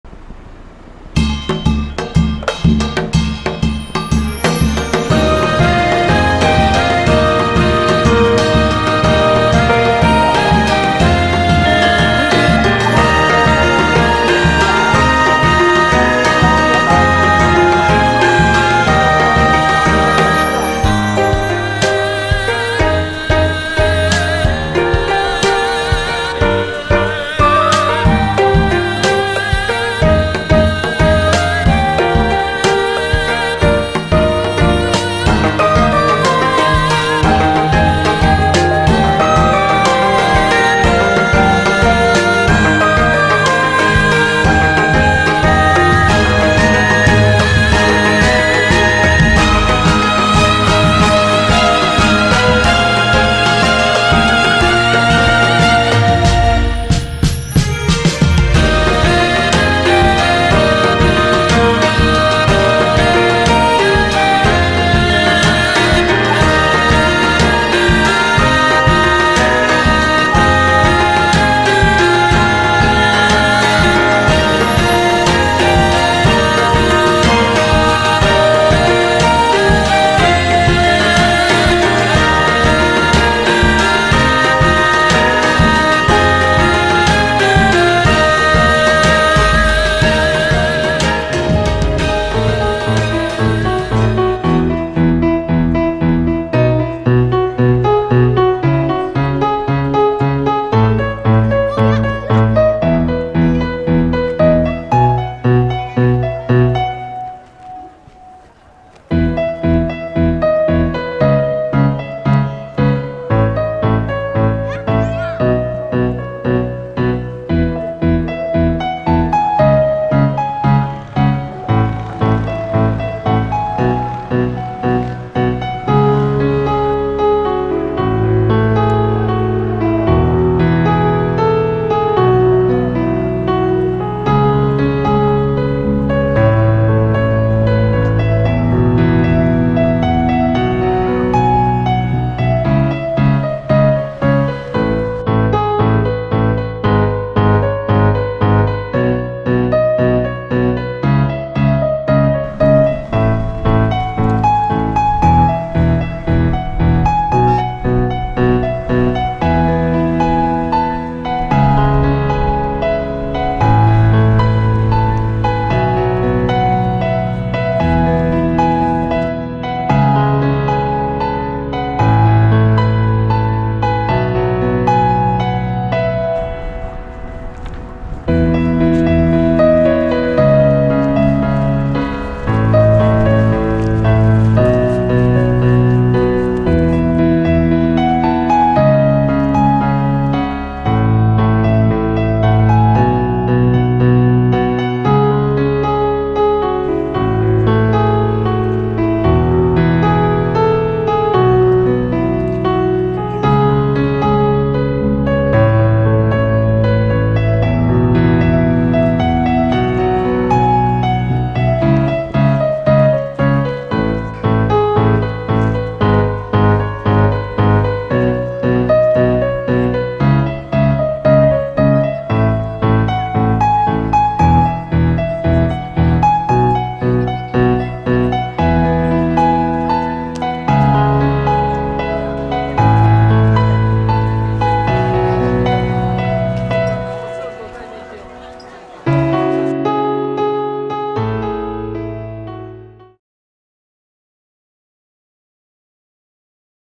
[DJI 로닌 RS3 미니] 수원 창룡문에서 동장대 까지 기록! 수원 화성 한바퀴
DJI 로닌 RS3 미니 수원 창룡문에서 동장대 까지 기록! 수원 화성한바퀴
사진의 모든것 카메라신